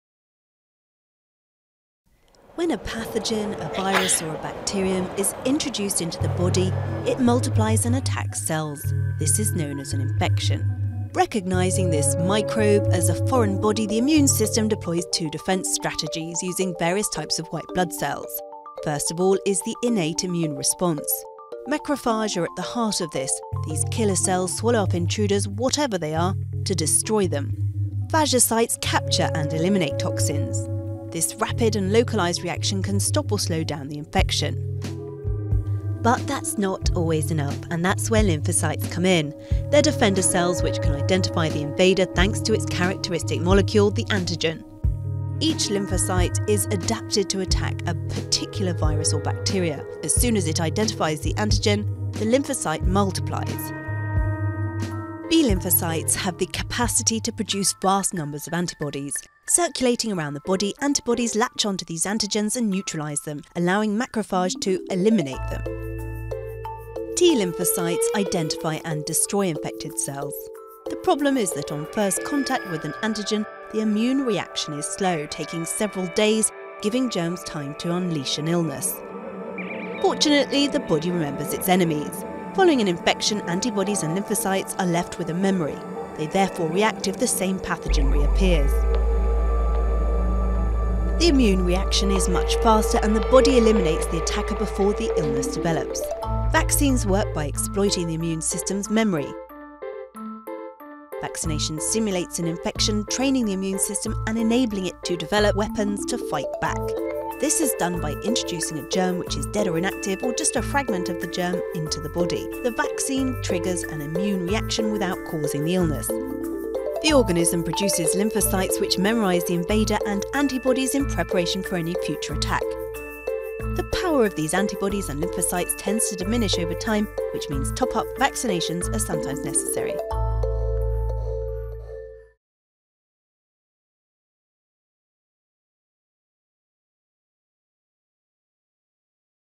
Vaccination
Vaccines work by exploiting the immune system’s memory. UNICEF has warned that conflict, complacency and the growing anti-vaccine movement threatens to undo decades of work to tame the disease measles.VIDEOGRAPHIC